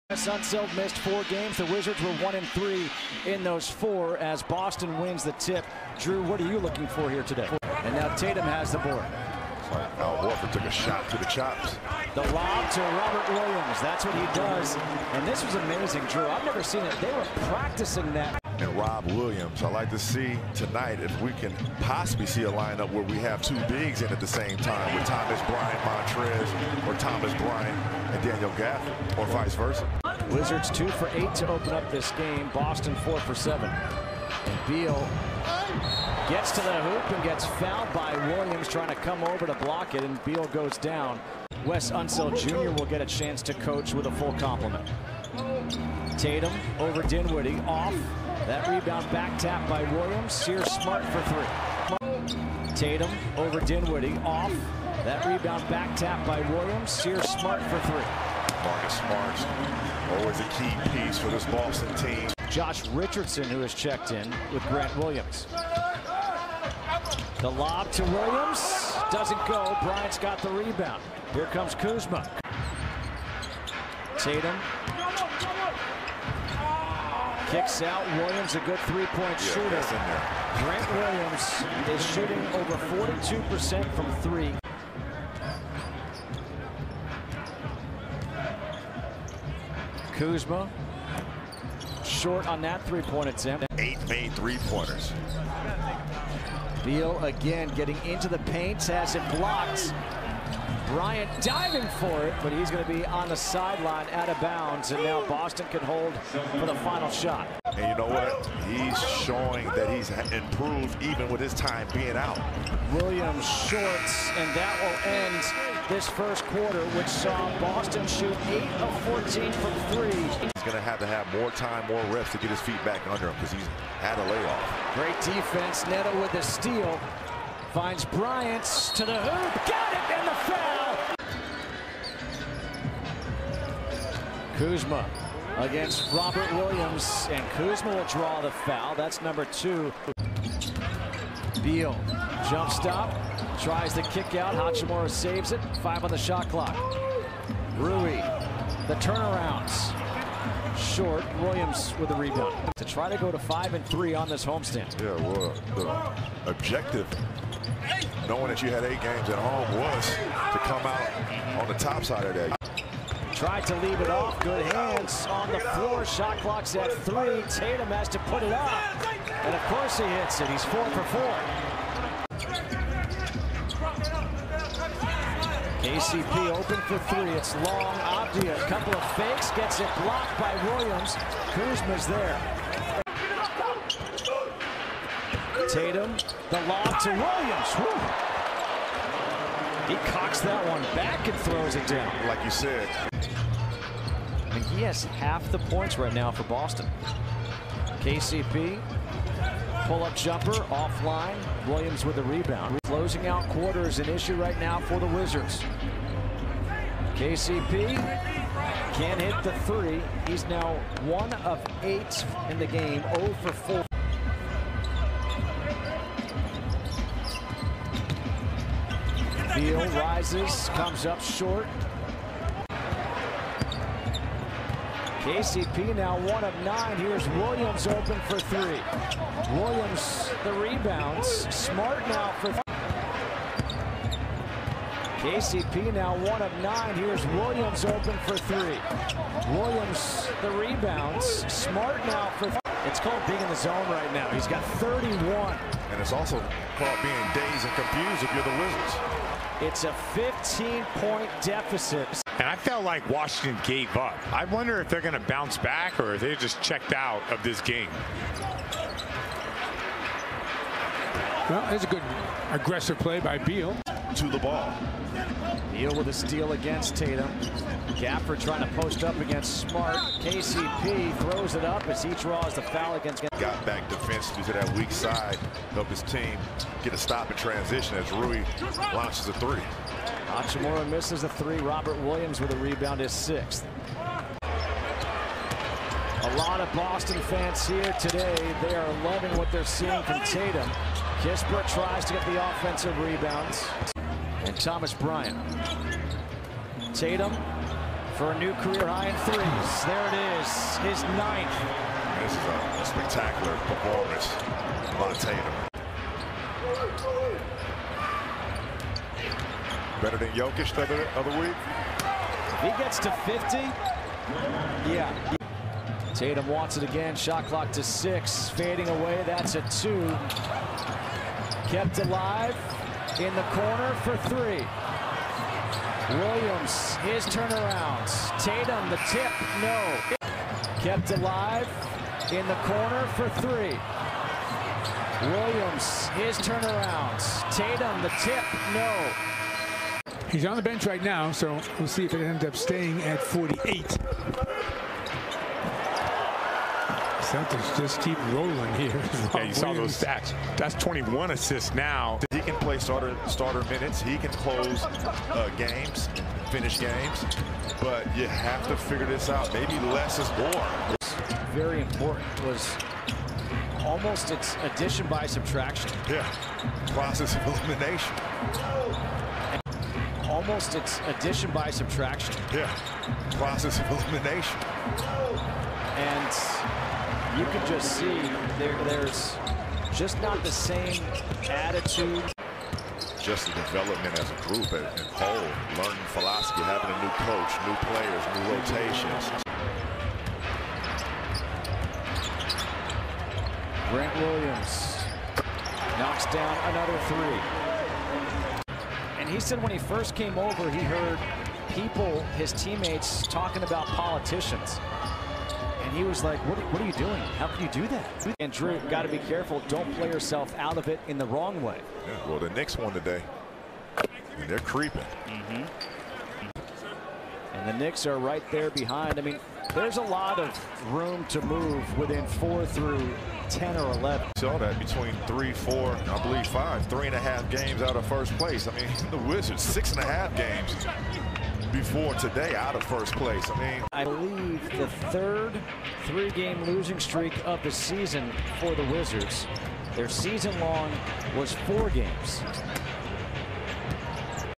Grant Williams 10 PTS 7 REB: All Possessions (2022-01-23)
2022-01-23 Washington Wizards vs Boston Celtics: Player Highlights Grant Williams
    10 PTS, 7 REB, 1 AST, 0 STL.
        2 of 3 from 3.
        3 of 6 FGs in 22:02 minutes.